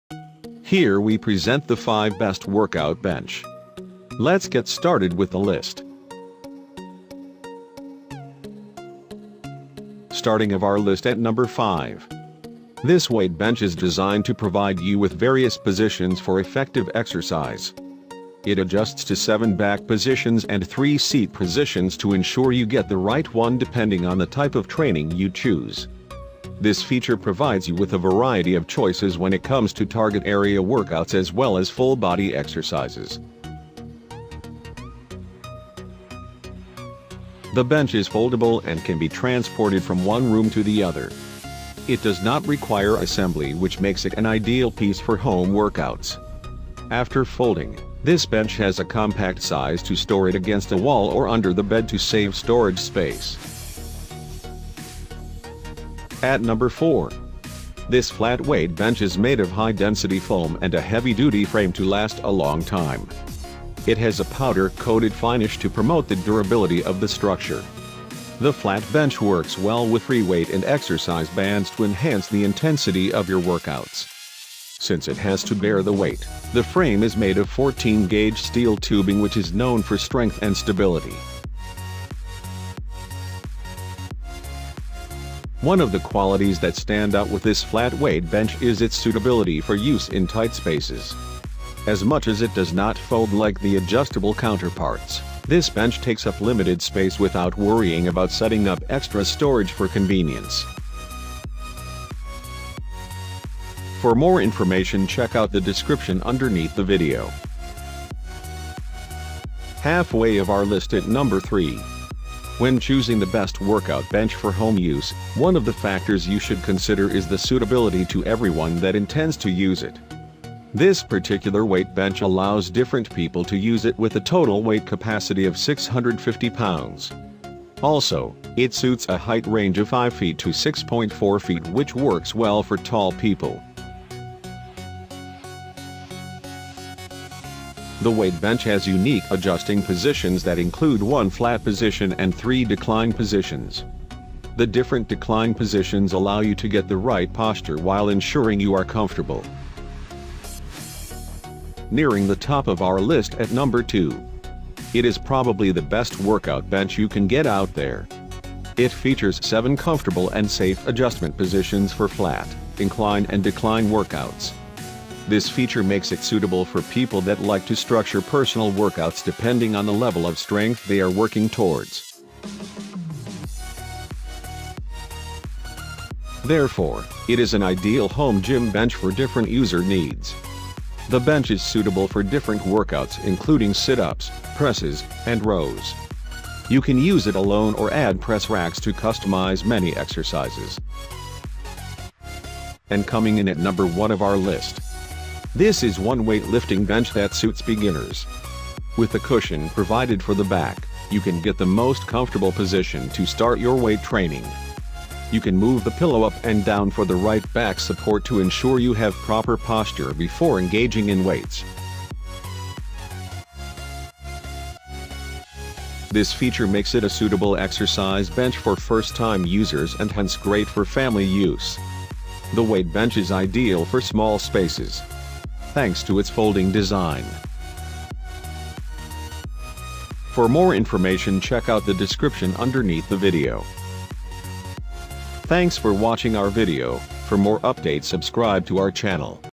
Best Workout Bench 2019 - 2020
Are you looking for the Best Workout Bench. We spent hours to find out the Best Workout Bench for you and create a review video. In this video review you will find the top products list, what is the feature of these products and why you should buy it.